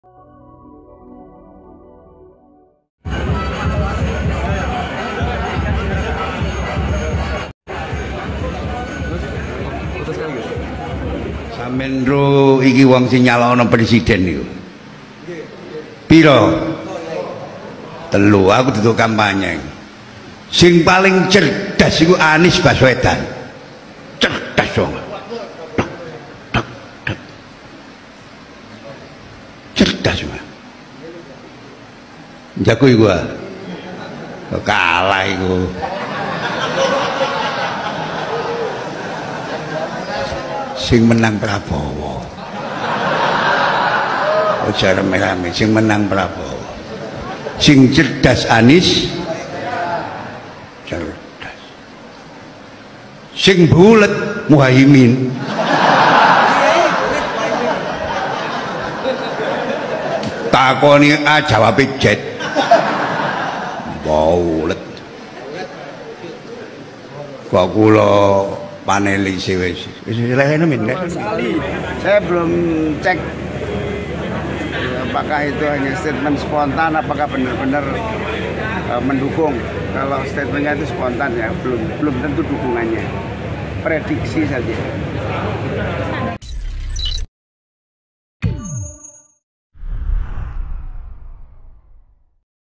Begini tanggapan Cak Imin tentang ceramah Gus Ali yang menyebutnya "mbulet"
Calon Wakil Presiden (Cawapres) nomor urut 1 Muhaimin Iskandar menilai ceramah Pengasuh Pondok Pesantren Bumi Shalawat Sidoarjo, Gus Ali Masyhuri, yang sedang viral di media sosial dan dirasa menyudutkannya merupakan pernyataan spontan. Menurutnya bukan pernyataan yang mendukung salah satu pasangan Calon Presiden di Pemilihan Umum (Pemilu) 2024.